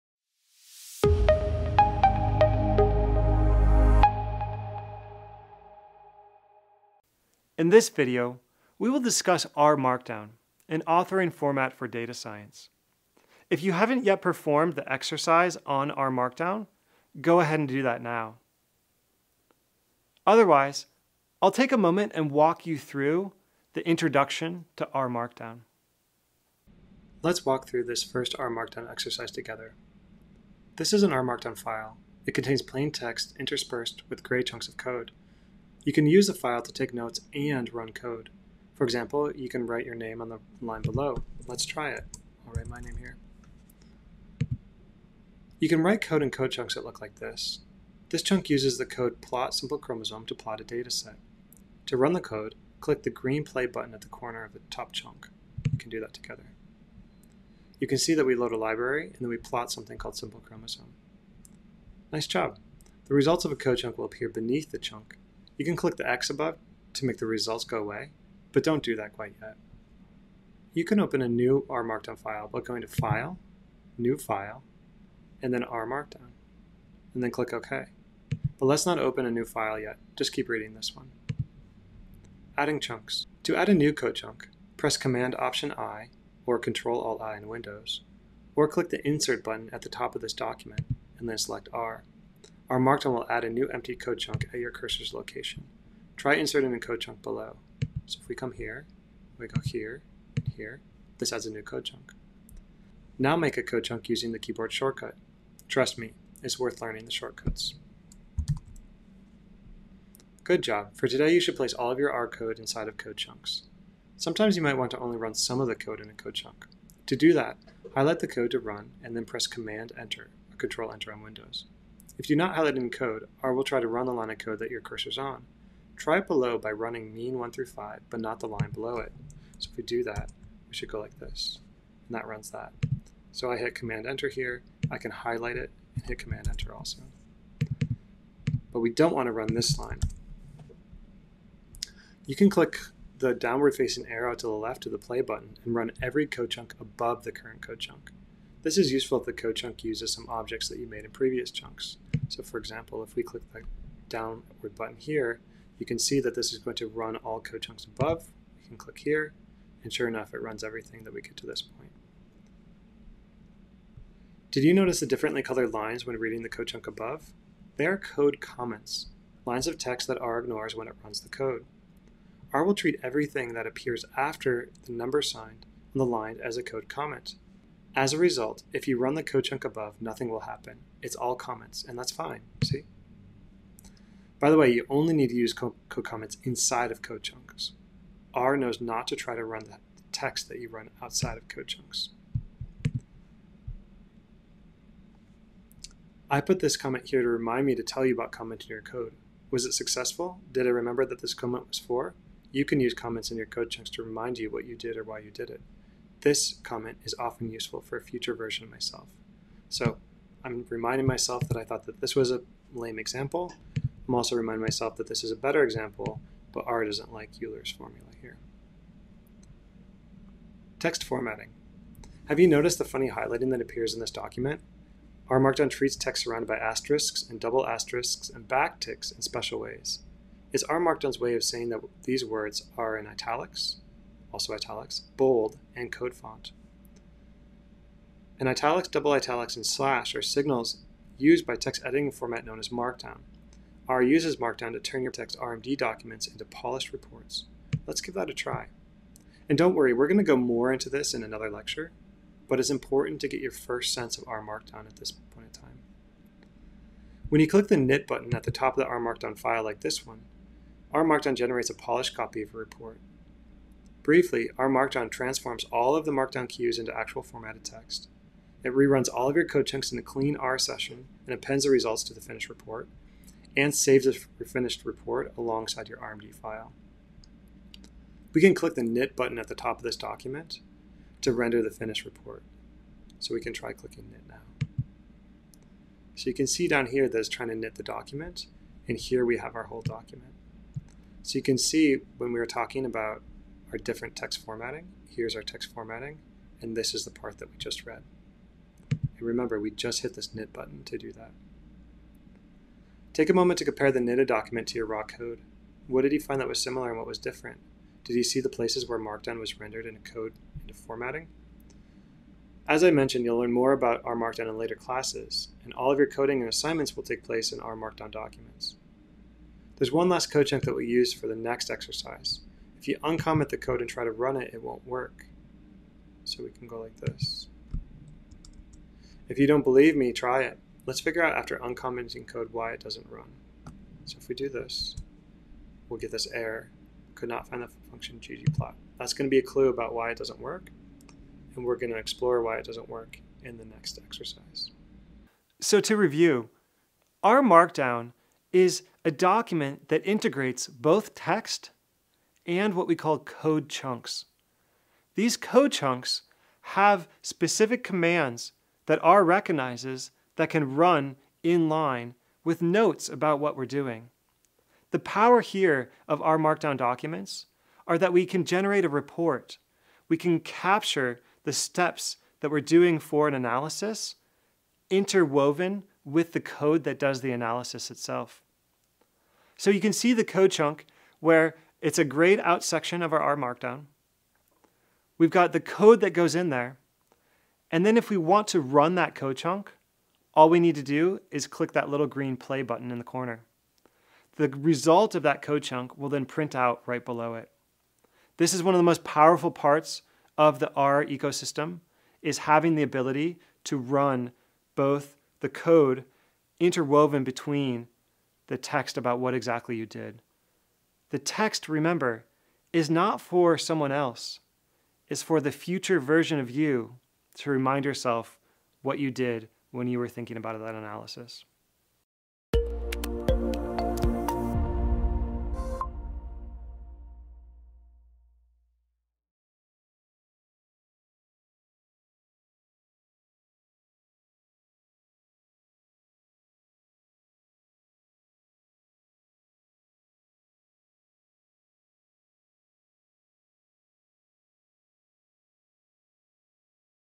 Introduction to RMarkdown to create reproducible reports
In this video, you will learn to reproduce and parameterize reports using R Markdown in R and RStudio. All you need to get started is the videos provided below and an R Studio programming environment. By the end of this module, you will have the skills needed to begin working with reports for a data set of your choice using RMarkdown in R and RStudio.